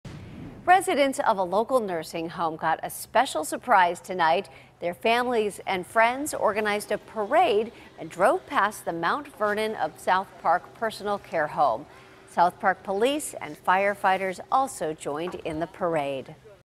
Family, Friends Host Parade At Mt. Vernon Of South Park Personal Care Home
South Park Police and firefighters also joined in on the parade.